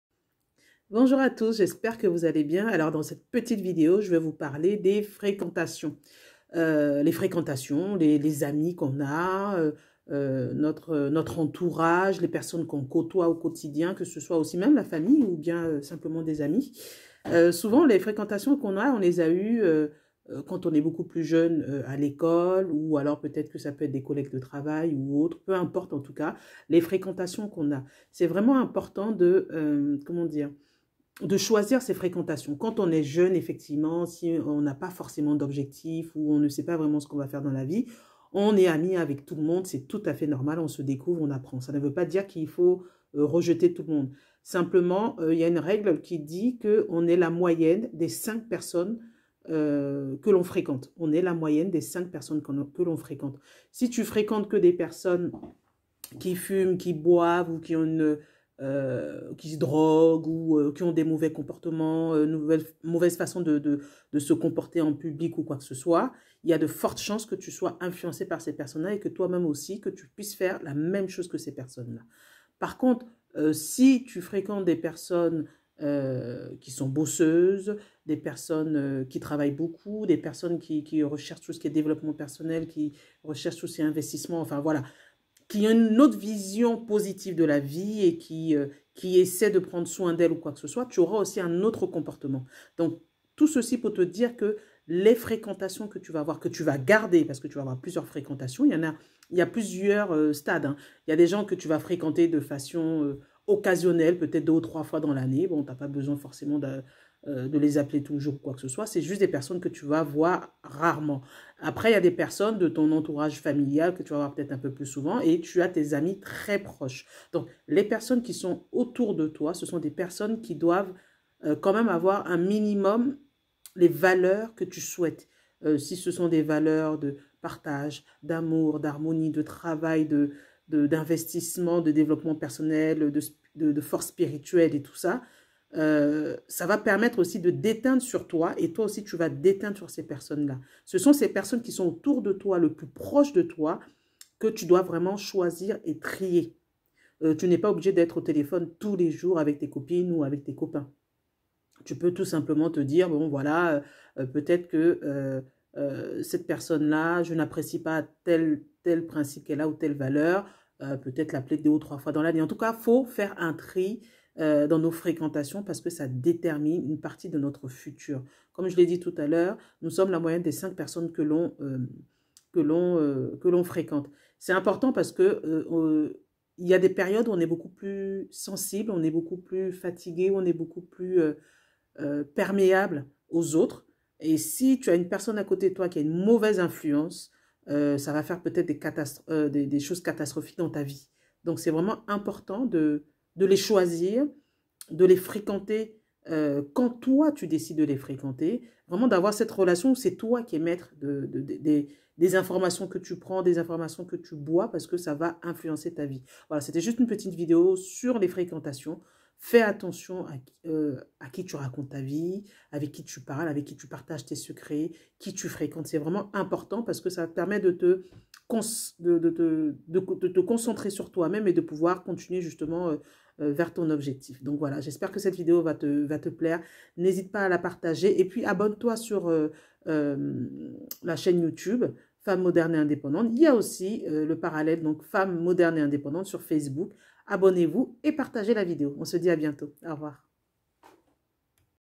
ATTENTION AUX FRÉQUENTATIONS!
Il est généralement reconnu que tu es la somme des 5 personnes que tu fréquentes. Les amis et proches sont à  choisir ou du moins choisir le type d'interactions que l'on souhaite avec eux. fais les bons, il sont déterminant pour toi.